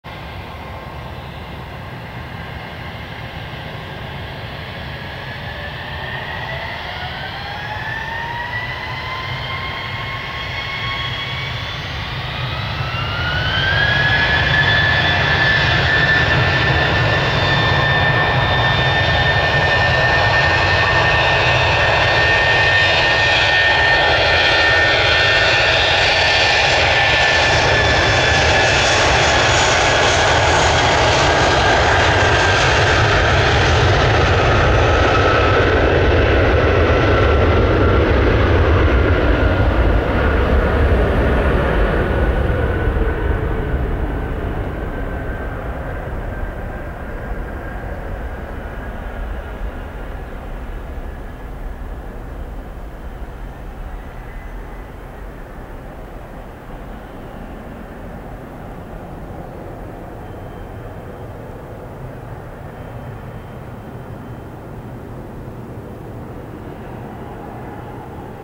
United Airlines N835UA A319 Takeoff Portland Airport (PDX)
Flight: UAL828 PDX-SFO